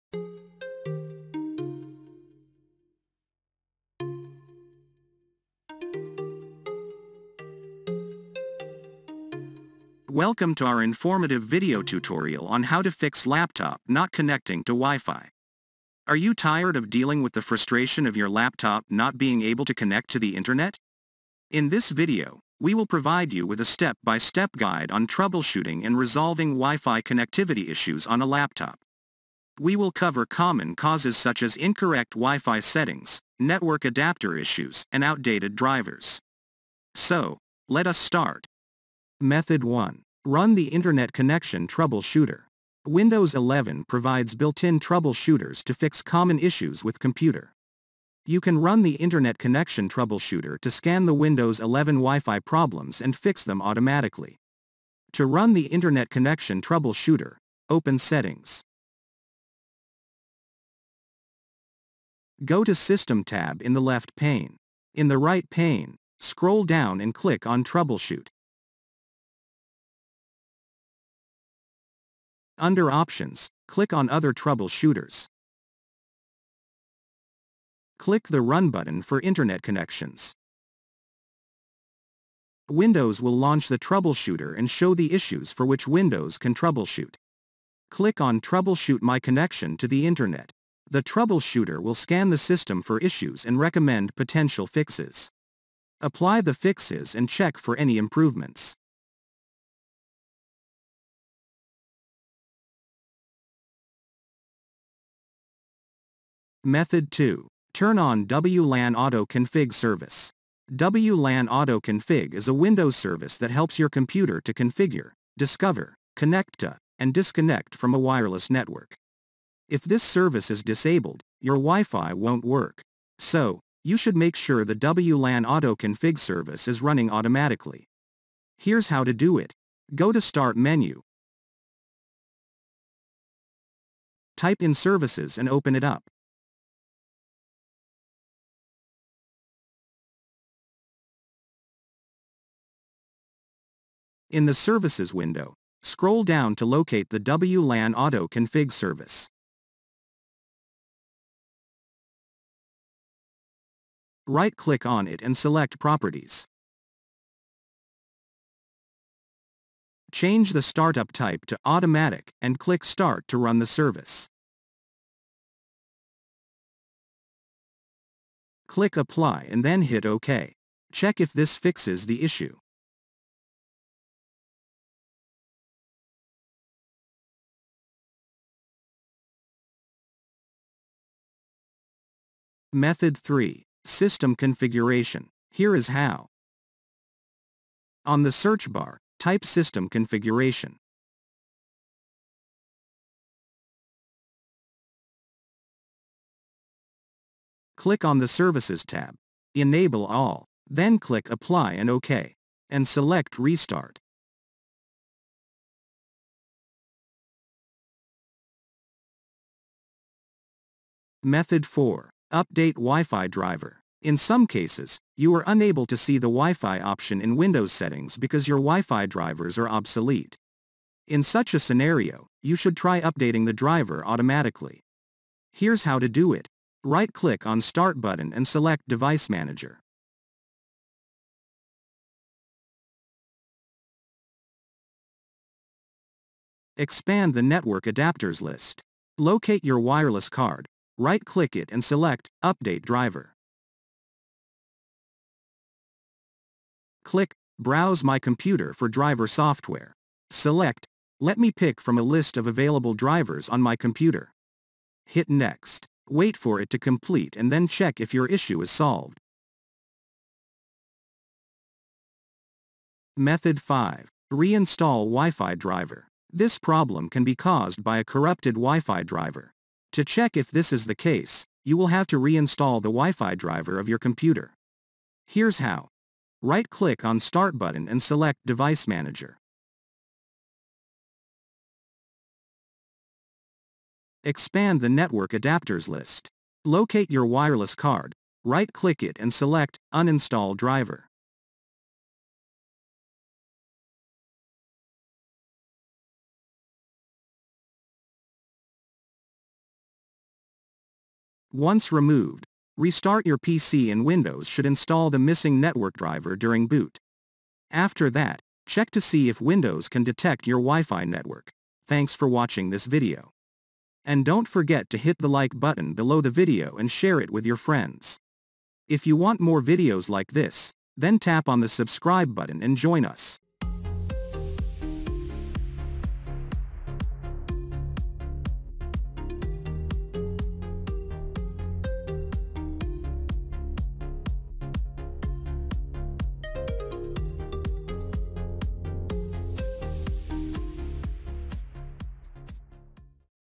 How to Fix Laptop Not Connecting to WiFi!! - Howtosolveit
Welcome to our informative video tutorial on how to fix laptop not connecting to wifi. Are you tired of dealing with the frustration of your PC laptop not being able to connect to the internet? Wifi icon not showing, no wifi networks found and ethernet connected but no internet access are windows wifi problems. In this video, we will provide you with a step-by-step guide on troubleshooting and resolving wifi connectivity issues on a laptop. We will cover common causes such as incorrect wifi settings, network adapter issues, and outdated drivers. We will also demonstrate advanced troubleshooting techniques such as resetting network settings, checking for interference from other devices, and updating your drivers. By the end of this video, you will have the knowledge and tools to fix your laptop's wifi connectivity issues and get back to using the internet seamlessly. So, watch this video and learn how to troubleshoot and fix "laptop not connecting to wifi" problem today.
For more video on how to get free wifi internet and how to free internet on phone.